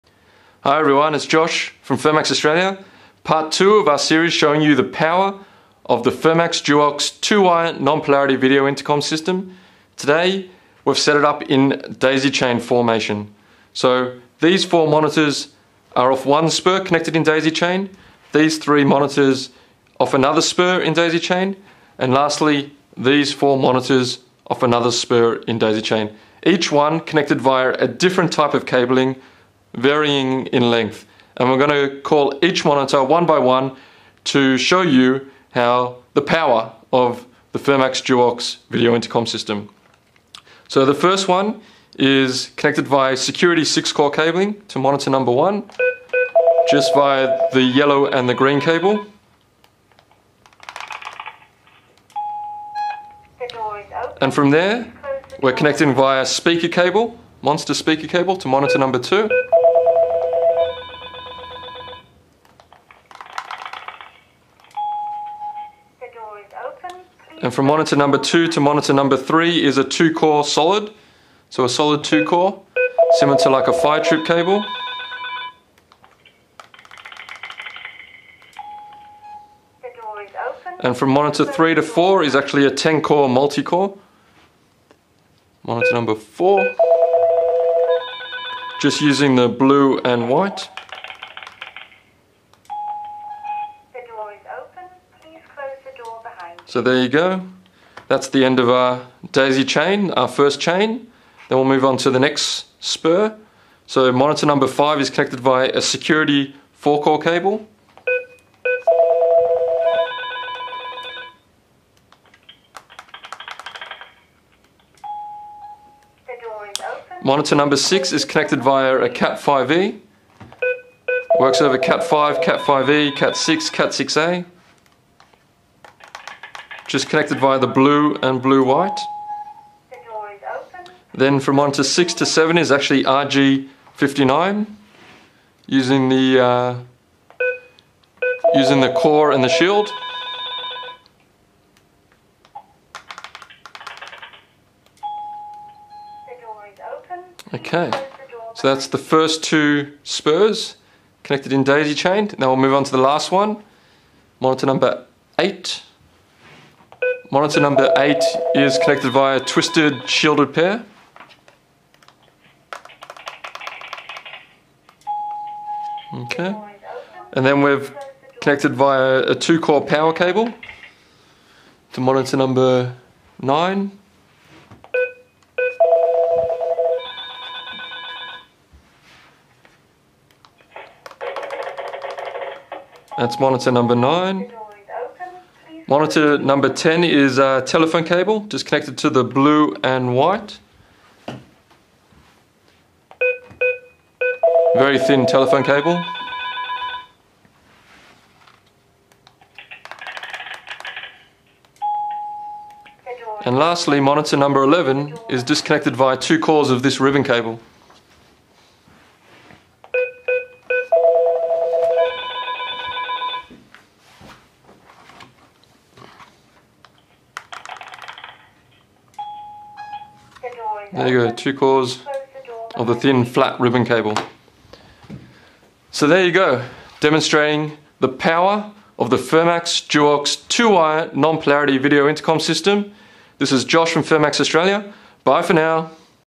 PART 2 - The Power Of The FERMAX DUOX 2-Wire Non-Polarity Video Intercom System
Demonstrating the power of the FERMAX DUOX 2-Wire Non-Polarity Video Intercom system working over different types of cables in the one system in daisy-chain configuration.